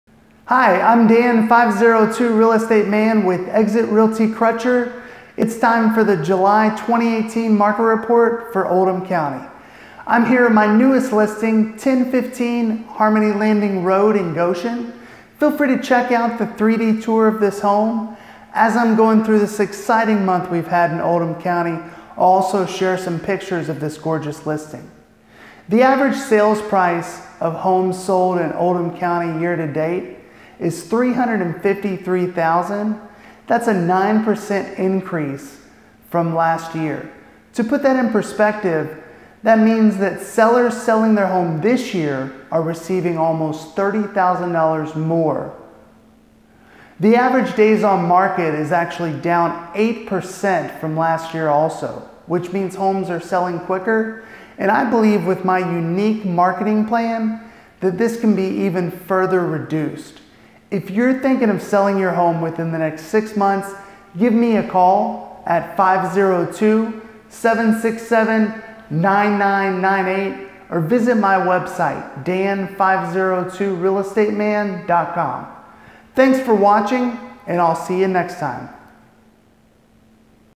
July 2018 Market Report Oldham County
This video gives a breakdown of the July 2018 Market Report for Oldham County and showcases my newest listing on 1015 Harmony Landing Rd Goshen, Ky 40026